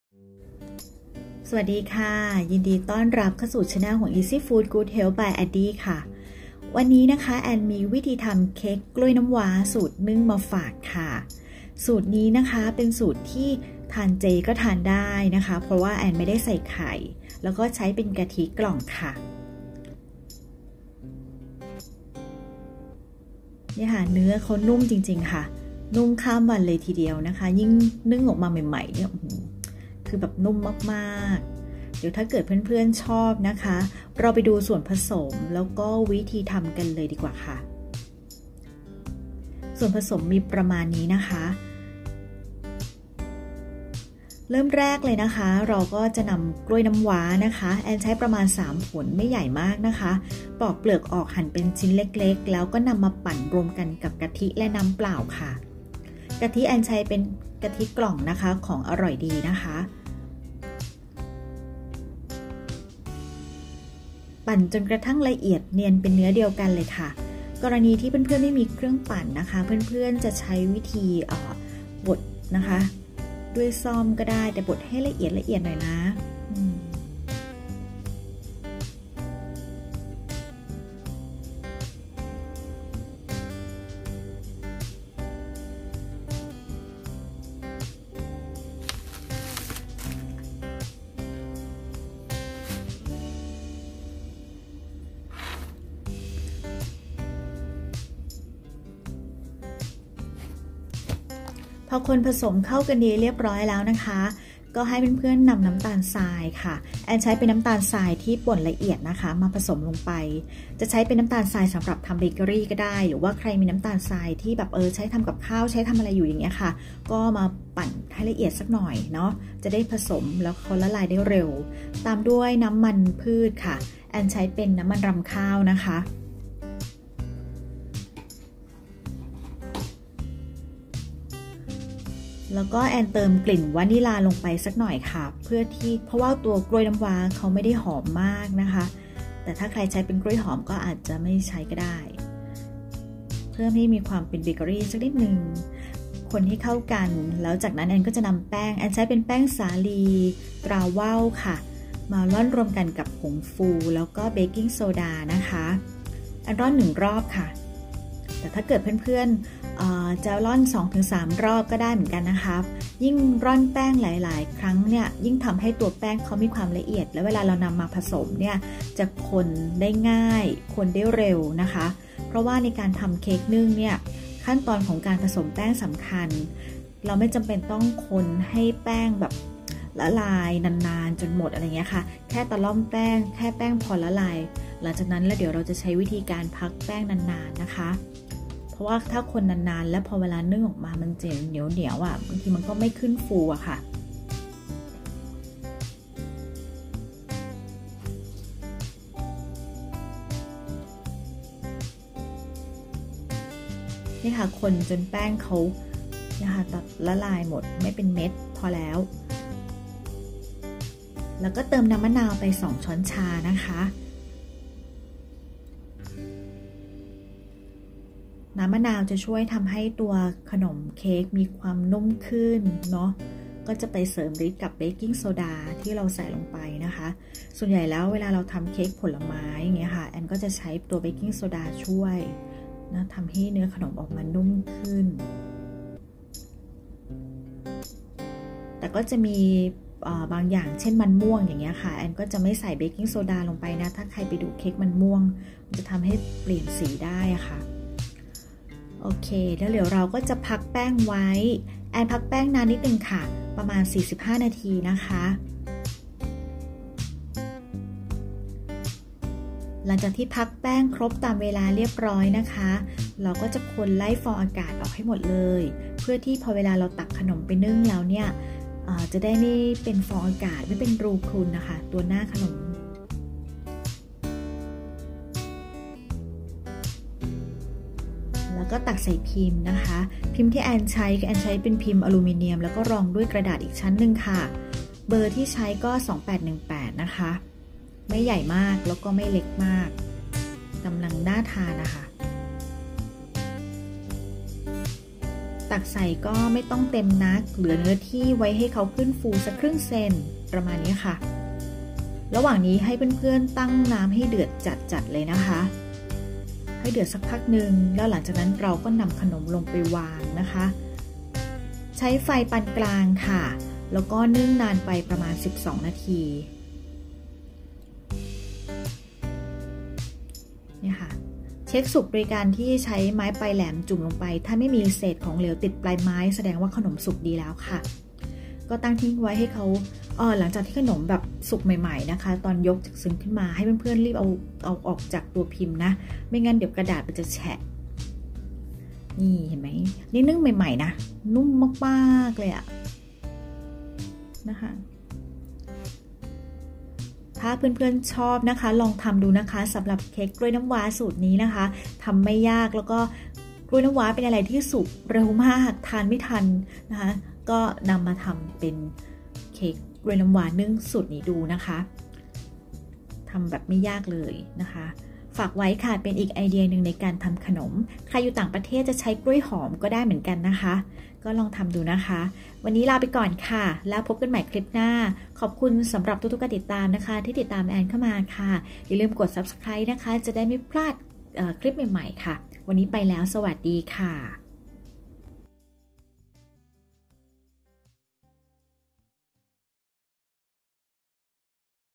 เค้กกล้วยน้ำว้านึ่ง สูตรเจ/Cultivate banana cupcakes no bake
คลิปนี้มาชวนทำเค้กกล้วยน้ำว้าสูตรนึ่ง
สูตรนี้ไม่ใส่ไข่ใช้กะทิ
ทำไว้ทานช่วงทานเจได้เลยค่ะ
เนื้อนุ่มมากๆ
ปรับเปลี่ยนปริมาณน้ำตาลตามความหวานของกล้วยได้เลยนะคะ

แป้งสาลีตราว่าว 1 ถต.
กล้วยน้ำว้า 3 ผล
กะทิกล่องอร่อยดี30มล.
น้ำเปล่า 60 มล.
น้ำมันรำข้าว 30 มล.
ผงฟู 1+1/2ชช.
เบคกิ้งโซดา 1/4ชช.
น้ำมะนาว2ชช.
Ingredients
1 cup.all purpose flour
3 Cultivate banana
100g.caster sugar
30ml.coconut milk
60 ml.water
30 ml.rice barn oil
1+1/2 tsp.baking powder
1/4 tsp.baking soda
2 tbsp.lime juice
1.ปั่นกล้วยน้ำว้า กะทิและน้ำเปล่าจนละเอียด
2.เติมน้ำตาลทรายบดละเอียดและน้ำมันรำข้าวคนให้เข้ากัน
3.ร่อนแป้งผงฟูและเบคกิ้งโซดาลงผสม ตะล่อมแป้งพอละลาย
จากนั้นพักแป้งไว้45นาที
4.เมื่อพักแป้งครบตามเวลา คนไล่ฟองอากาศ ตักใส่พิม
ตั้งน้ำให้เดือด
นำขนมไปนึ่งใช้ไฟปานกลางนึ่งนาน 12-15นาที
แอนใช้พิมกระดาษและพิมพ์จีบเบอร์2818
ทำได้ประมาณ9ถ้วยค่ะ
ลองทำดูนะคะ
ชมเมนูอื่นๆเพิ่มเติม⏬

เข้าไปพูดคุยกันได้นะคะที่แฟนเพจ